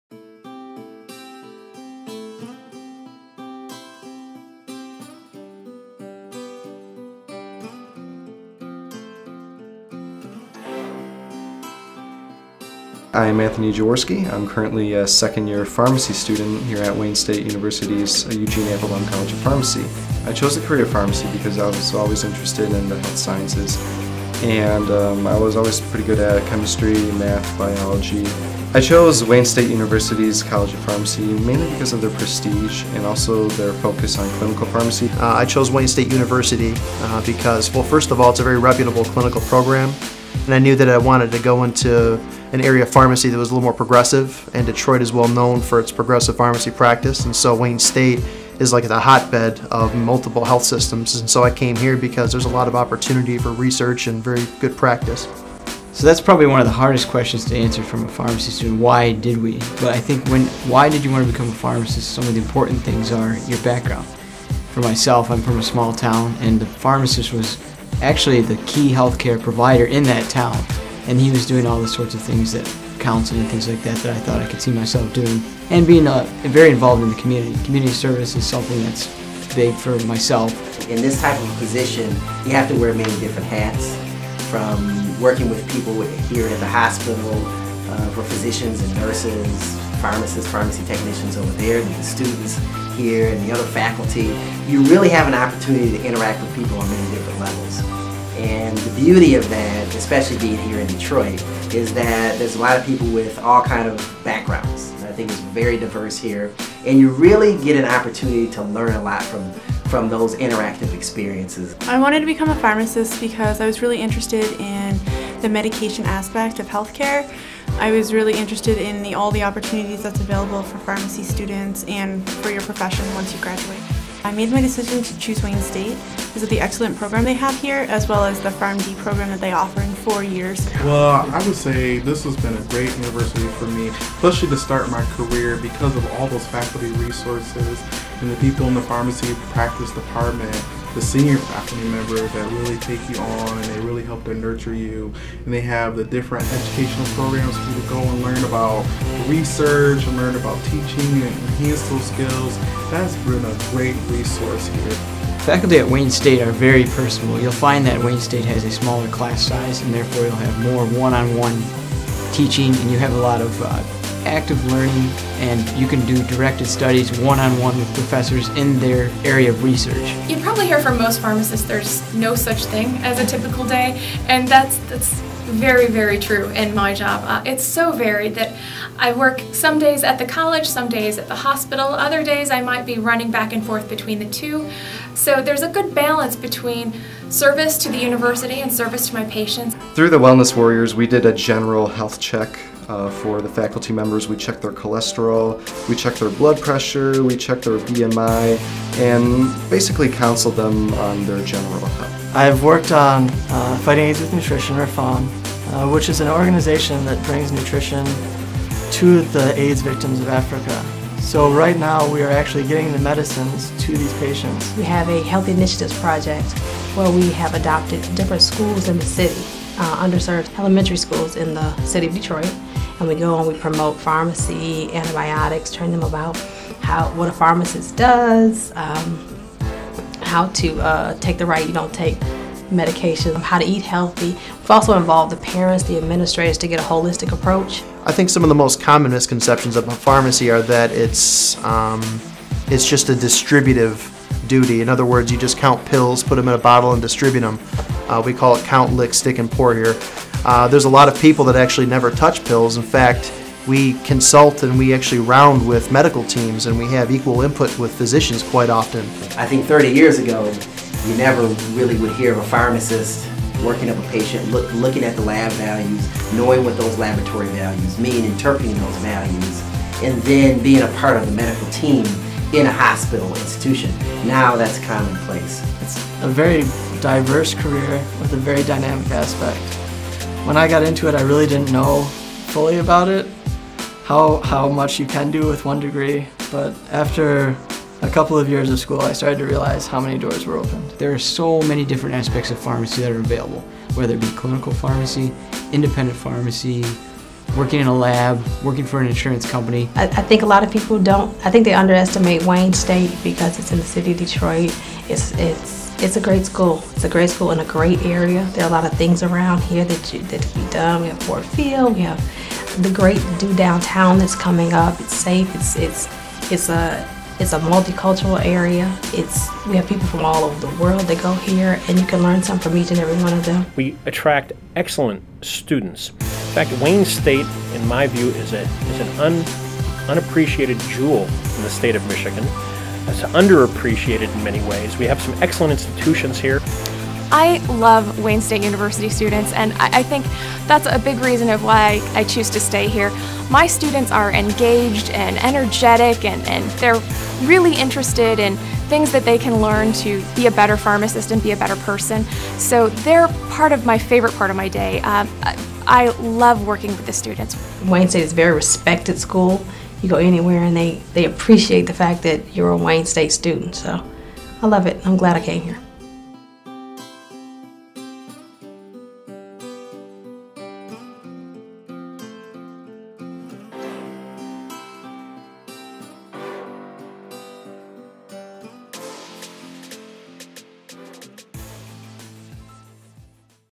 Study Pharmacy at Wayne State University
The Wayne State University Pharmacy program is located in one of the world's leading urban research universities. We educate future pharmacists to be highly competent health care practitioners who effectively and safely provide patient-centered care with other health care team members in diverse settings, to be leaders within the profession and to be lifelong learners.